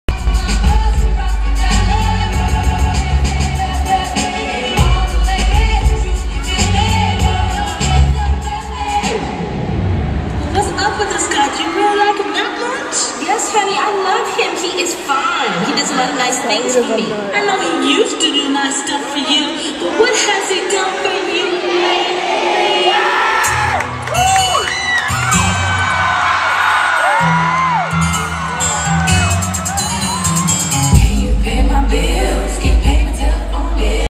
Female dancers
Beyonce @ Palace of Auburn Hills July 18th 2009. Anytime the video starts to shake or it ends suddenly it's because I started dancing or stopped recording so that I could do the routine. :)